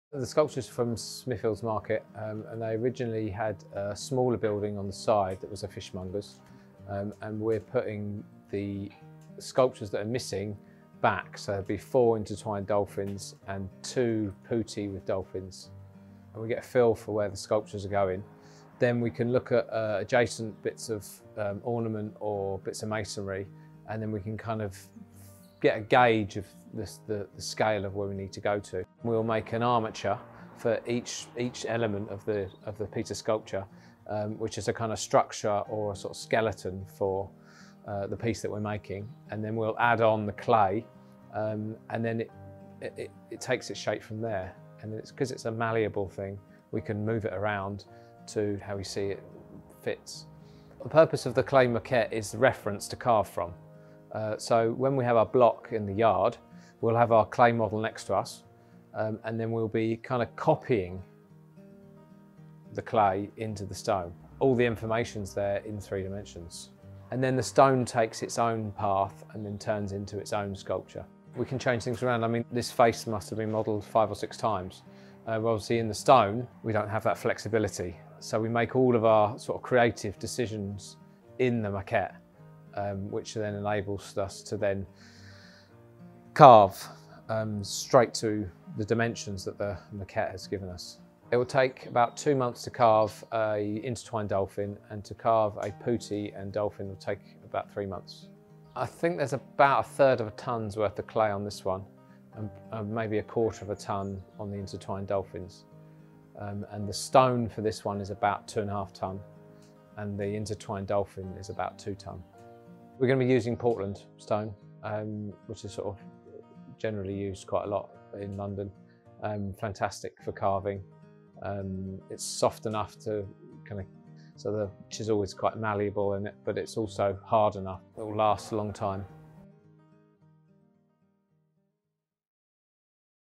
Recreating Smithfield Fish Market Sculptures
Working with Sculptor Ivan Cudby, PAYE Stonework are replacing lost and damaged sculptures of putti riding fish over the entrances at the Smithfield Fish Market, as well as entwined fish sculptures on the four corners of the Snow Hill corner tower. Here we follow the creation of the clay maquette ahead of the stone carving...